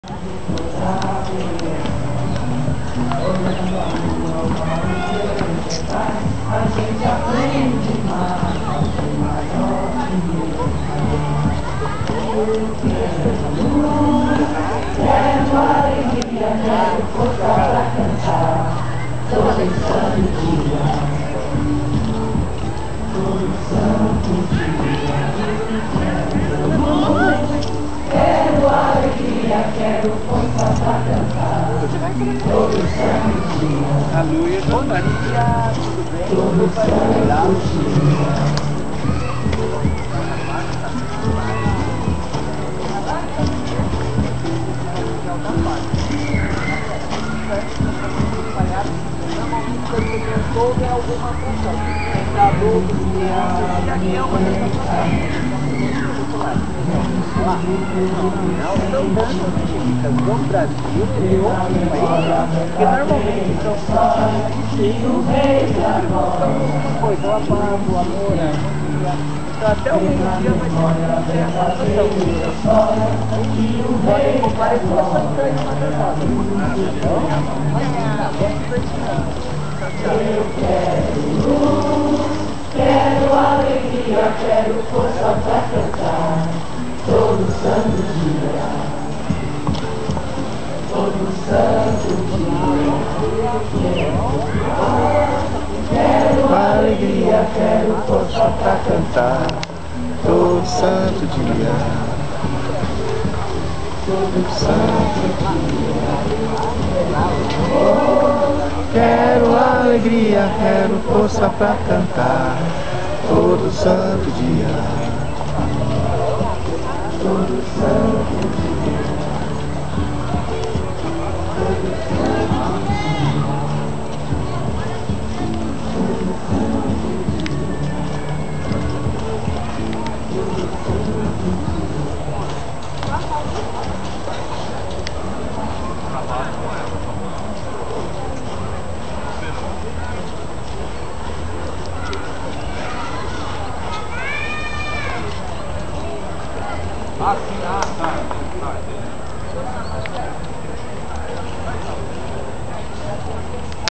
3º FestPAZ Ibirapuera Adriano Galhardo focaliza dança circular
O Professor e Design em Sustentabilidade - Adriano Galhardo - focaliza dança circular, com muita alegria, durante o 3º FestPAZ no Parque do Ibirapuera - 08 de setembro de 2012.